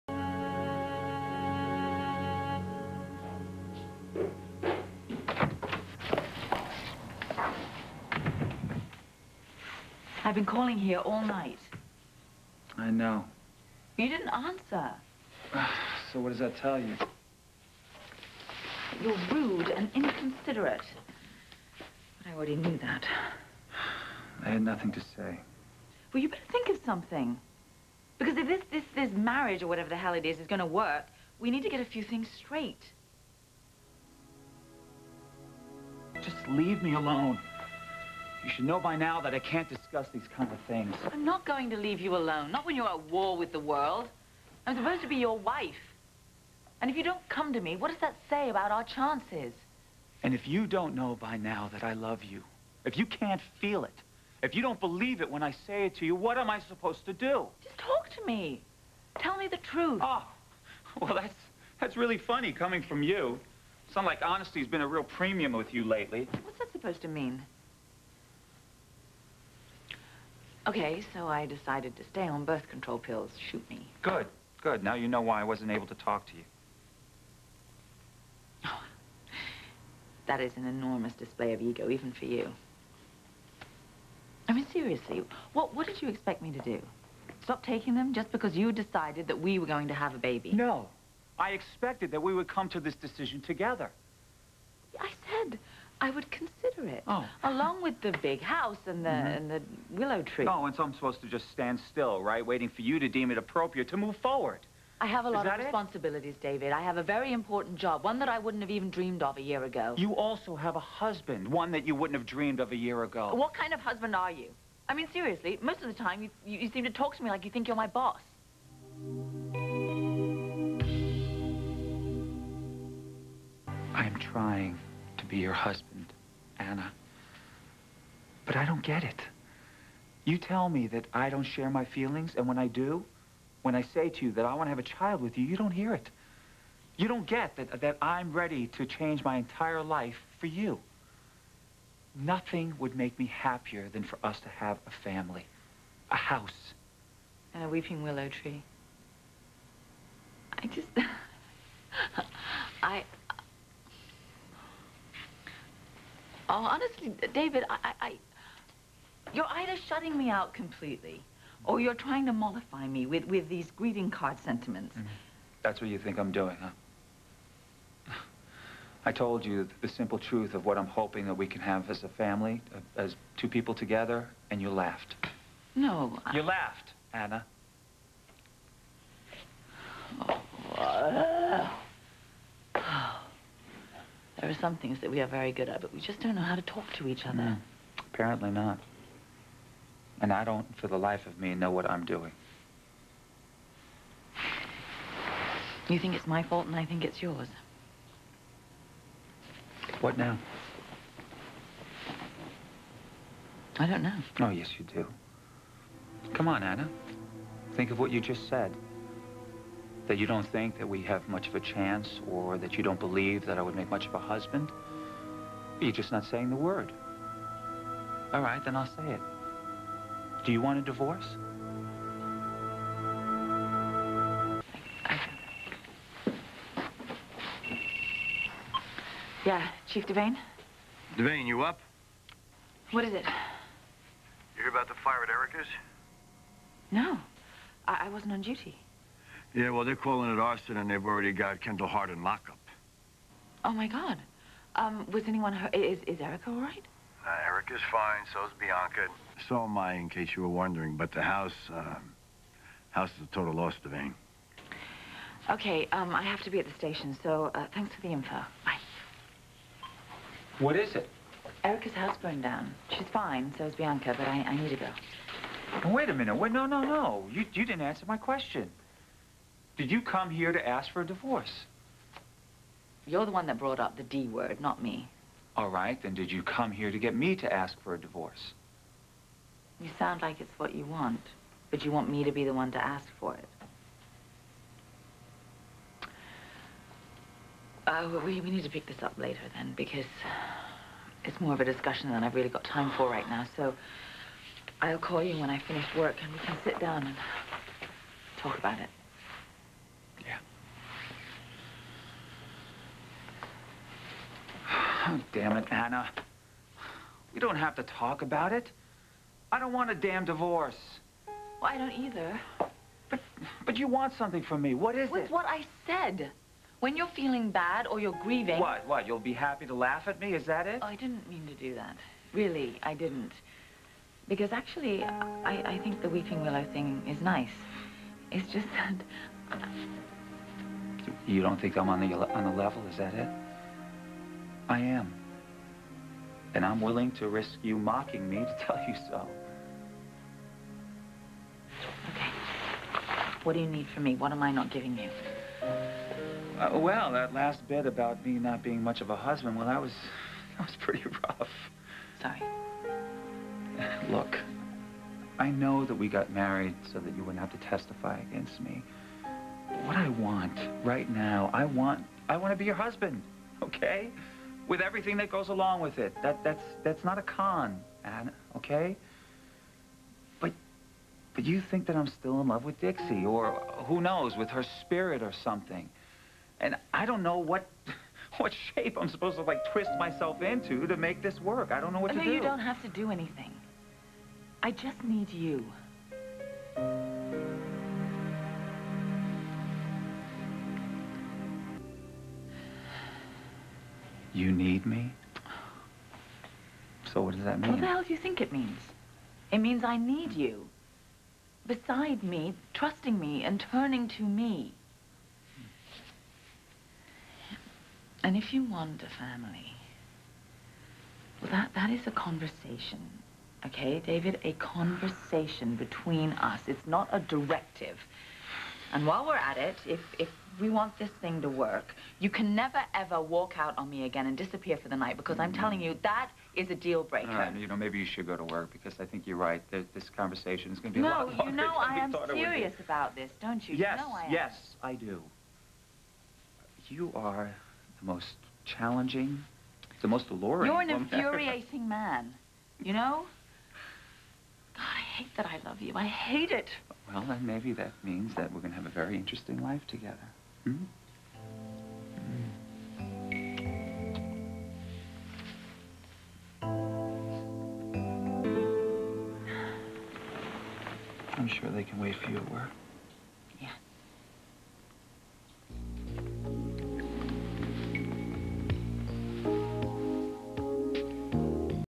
I Hate that I Love You
David spends the night at the cabin but Anna tracks him down. They have an intense conversation about getting a divorce.
5-21-02